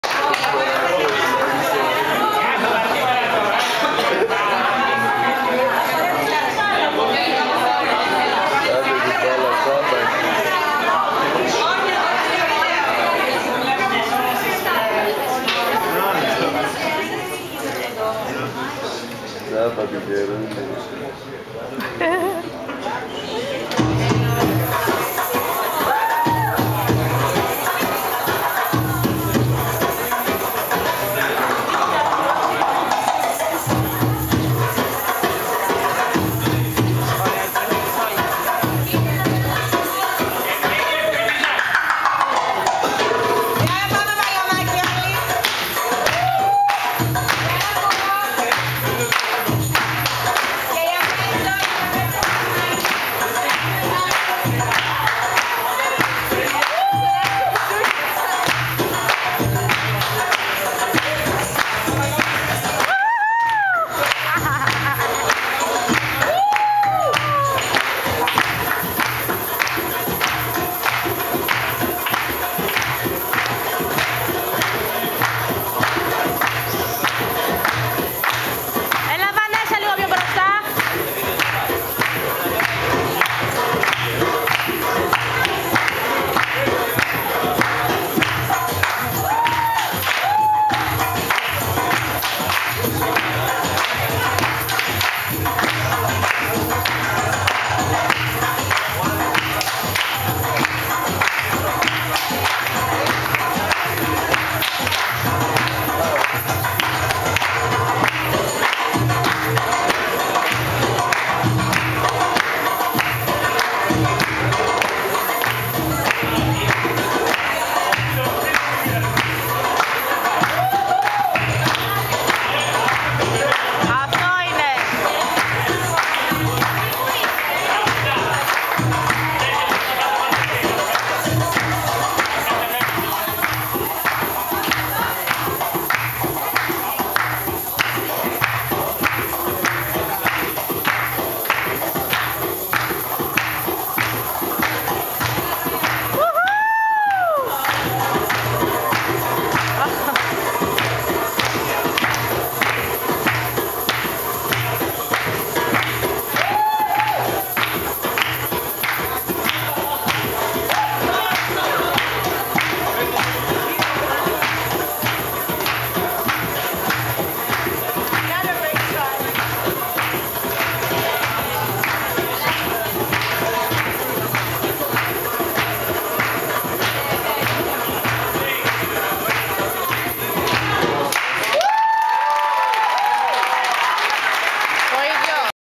DRUM SOLO !!!
DRUM SOLO COREOGRAPHY FROM MINA PANAGIOTOPOULOU AT TEATRO CAFE VESO MARE PATRAS...